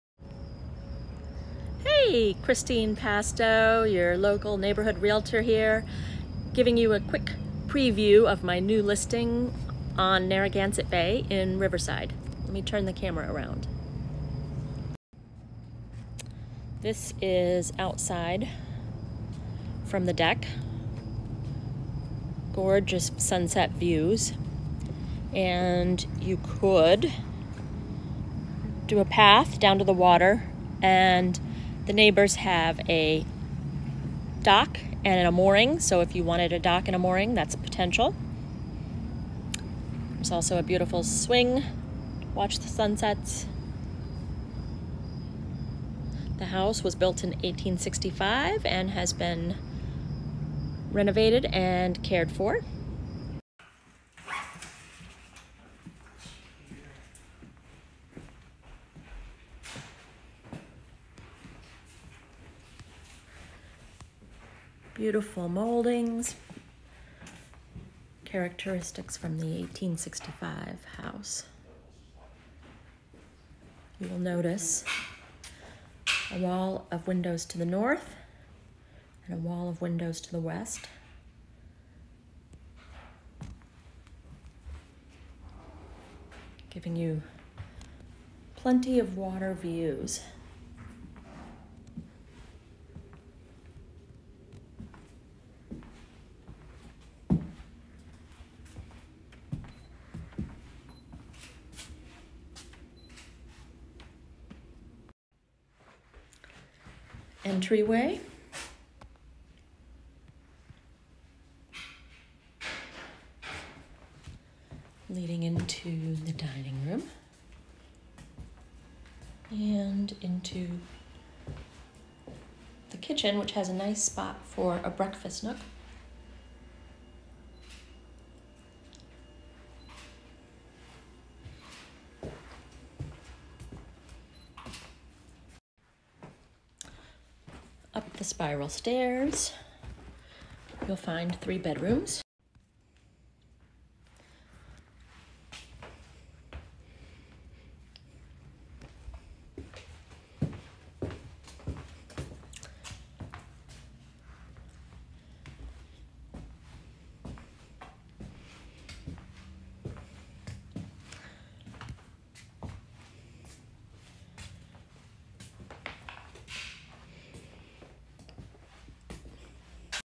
Sneak Peak of my new waterfront listing in RI
Coming next weekend is a three bedroom one and a half bath home on Narragansett Bay in Riverside.